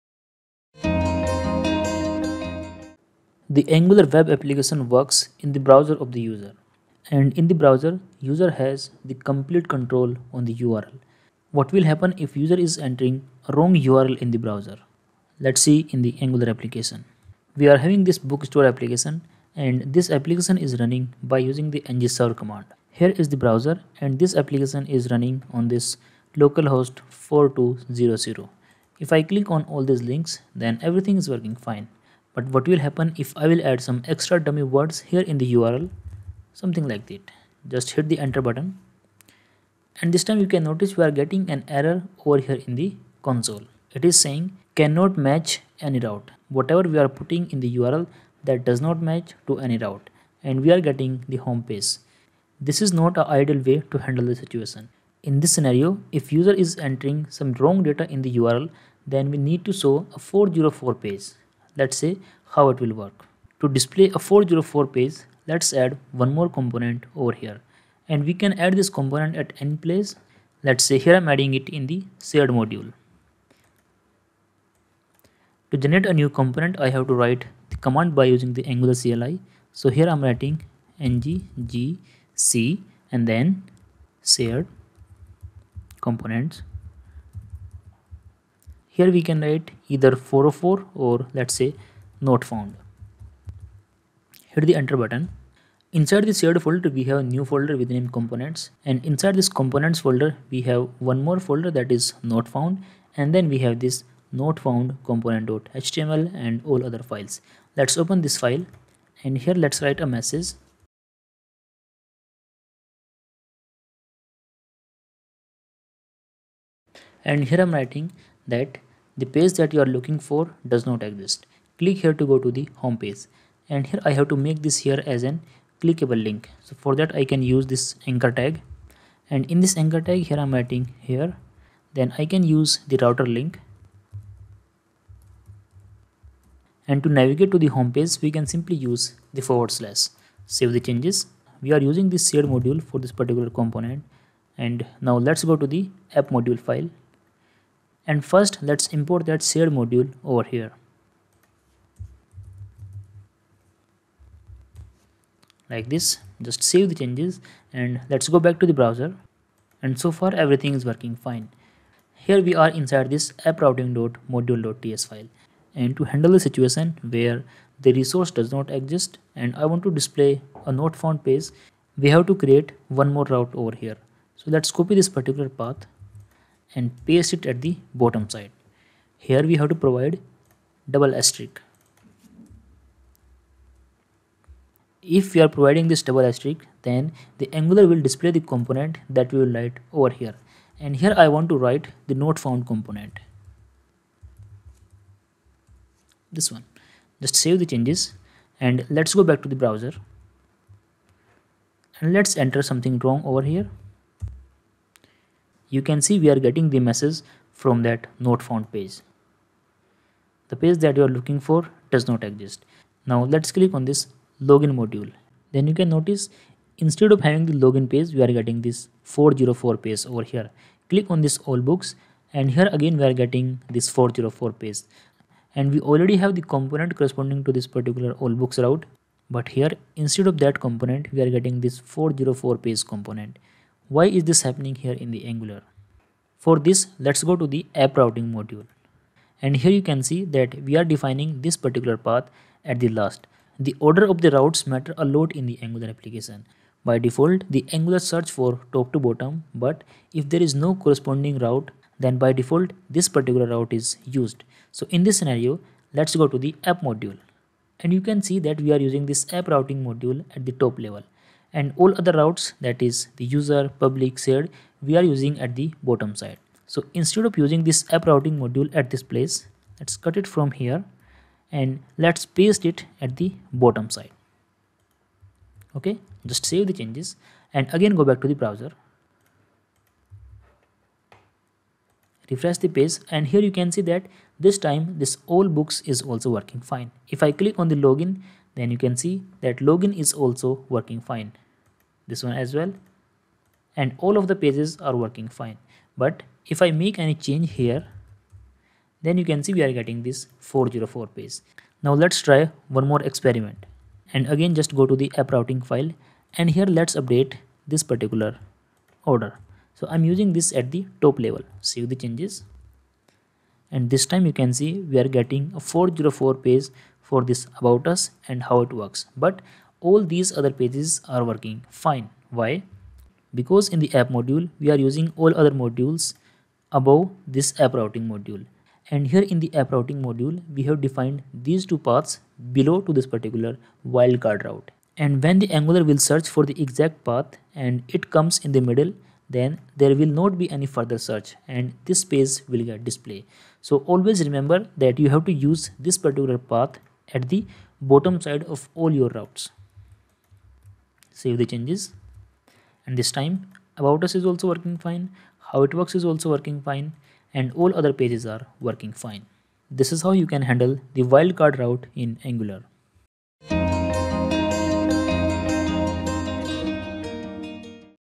Angular 404 page not found| Angular Tutorial
404 page in angular is used to display page not found information when the given URL does not match with any page in the application or the user enters a wrong URL in the browser.